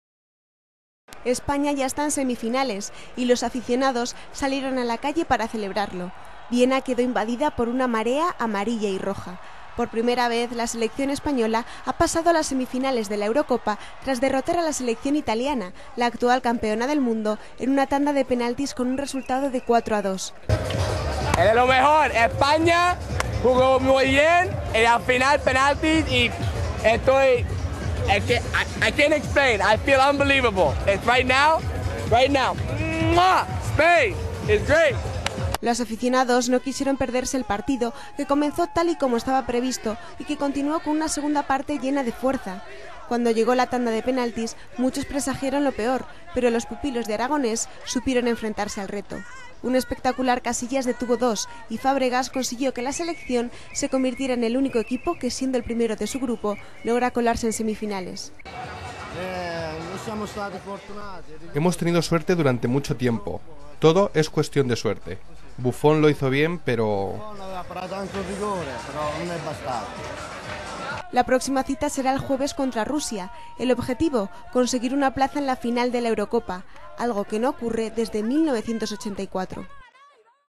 España cambia su historia en los cuartos
España cambió el rumbo de su historia en los cuartos de final y accedió a las semifinales de la Eurocopa, después de derrotar en la tanda de penaltis, por 4-2, a la actual campeona del mundo, Italia, tras el empate a cero con el que se llegó al término del encuentro y la prórroga.